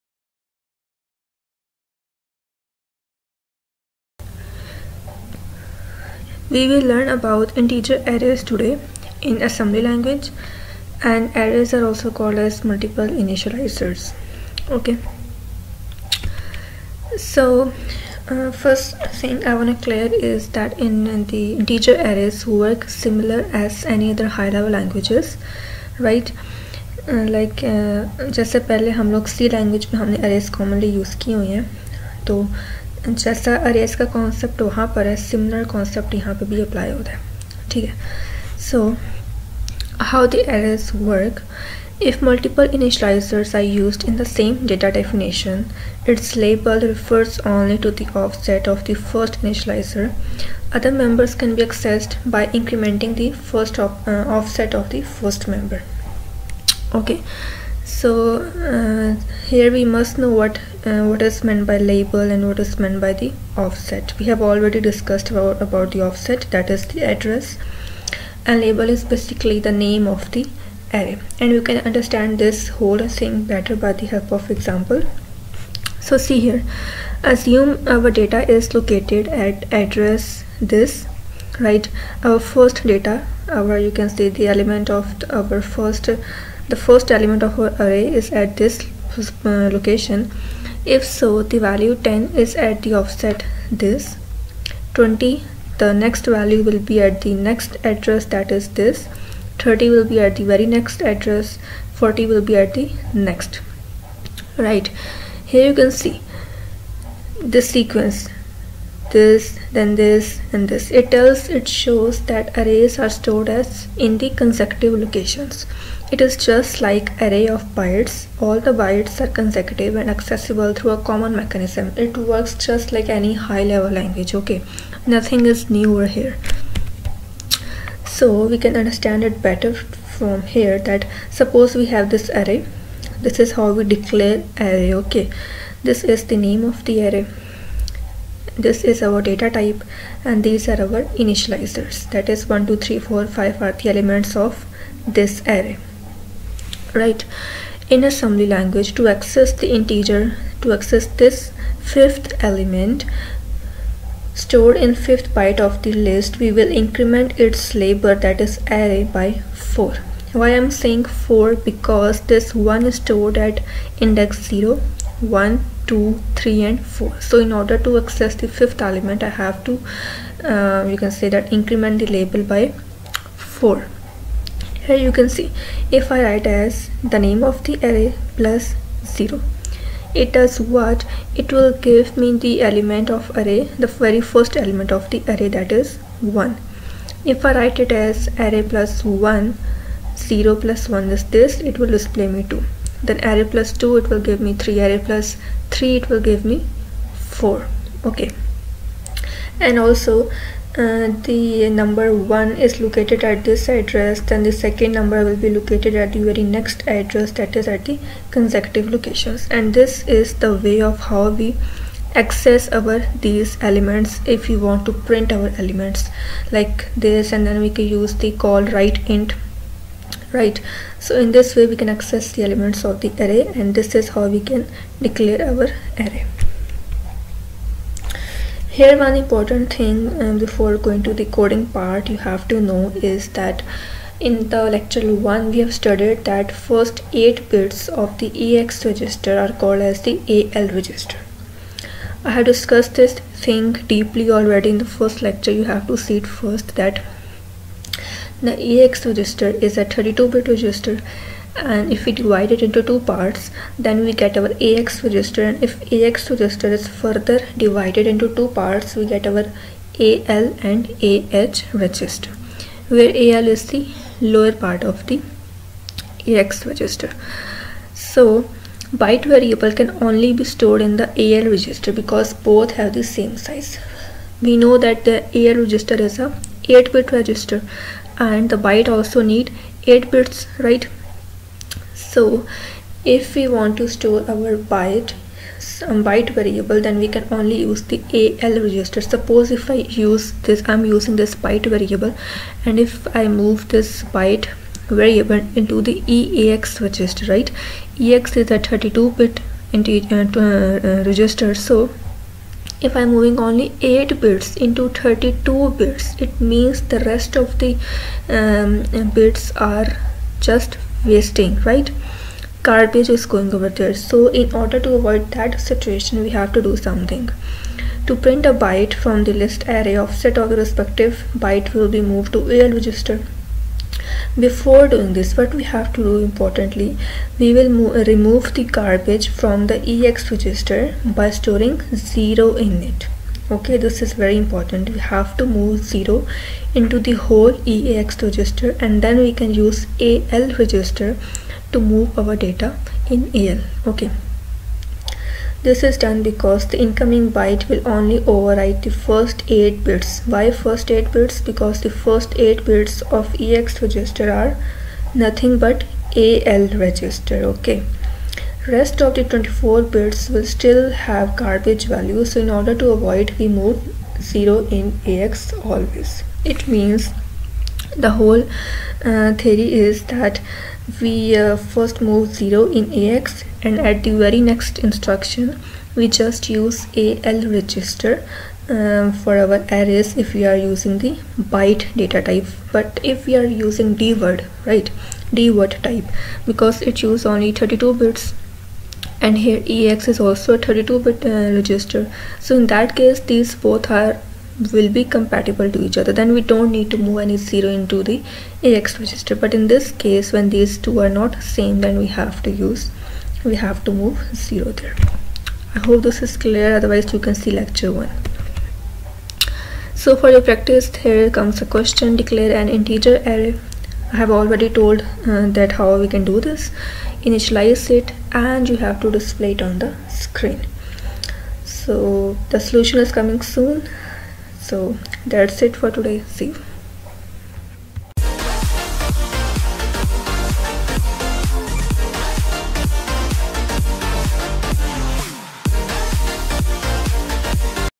Integer Arrays in x86 - Assembly x86 - Theory
This video provides an in-depth explanation of integer arrays in Assembly x86.